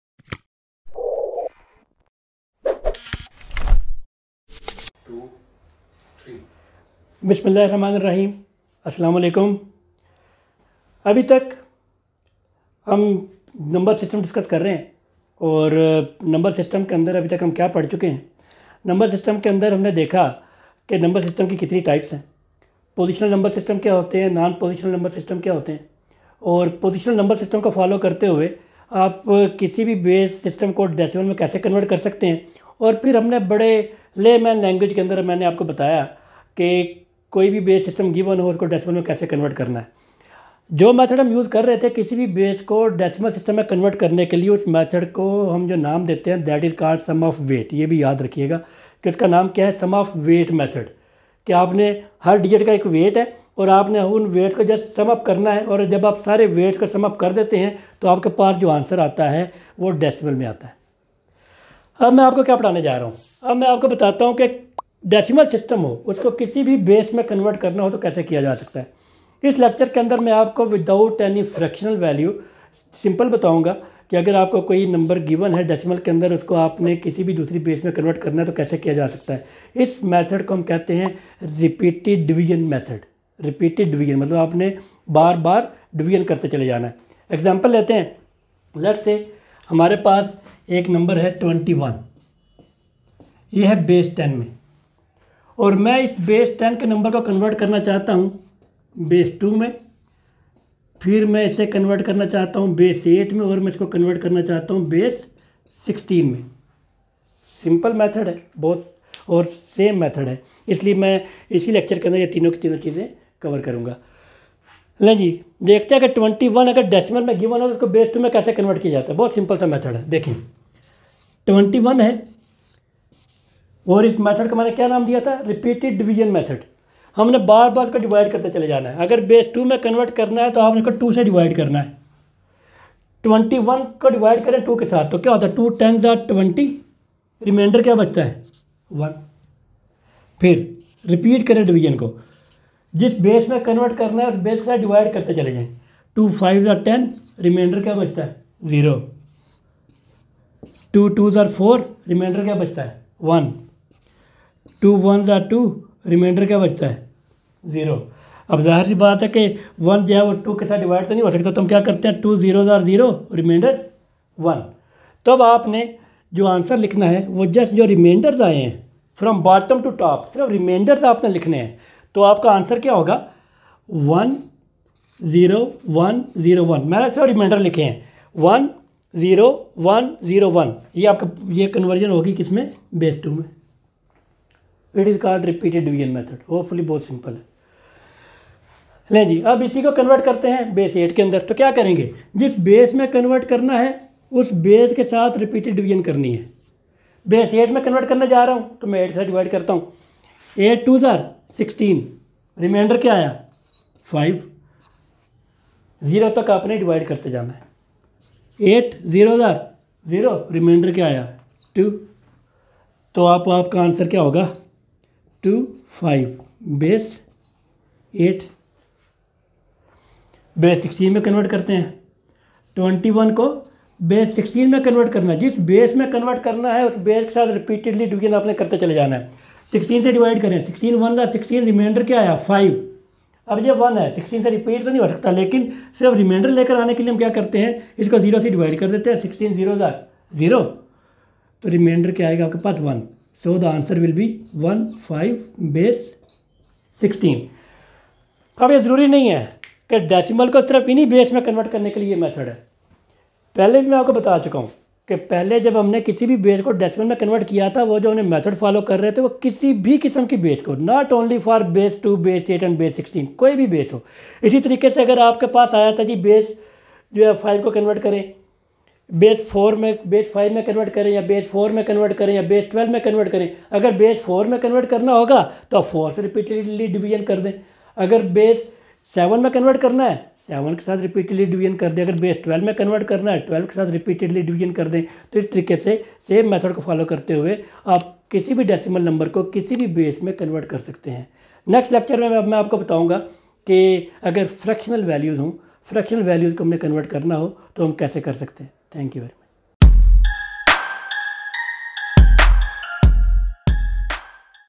Lecture 6 Part 1 | Convert decimal base system to any base system in Urdu/Hindi
In this video you will learn how to convert any number given in decimal base system to any other base system.
DecimalToBinary
DecimalToOctal
DecimalToHexaDecimal

Convert any decimal number to any other base system in urdu/hindi by Dr Usman Hashmi.

What is Number System?